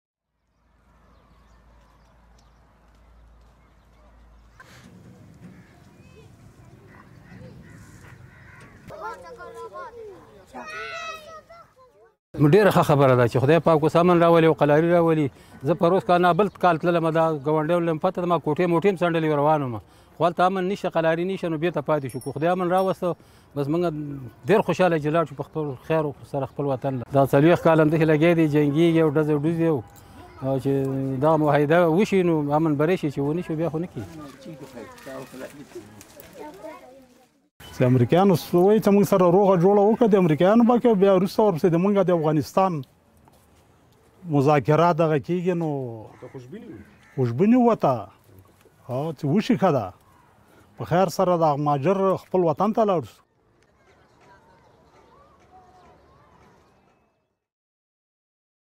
Refugees in Pakistan hopeful for end to conflict | AFP
Afghan refugees who have been living for years at the Khazana Refugee Camp in Peshawar, Pakistan are hopeful that the new US-Taliban peace deal can ultimately bring peace and allow them to return to their home country, after a nearly two-decade long deadly conflict.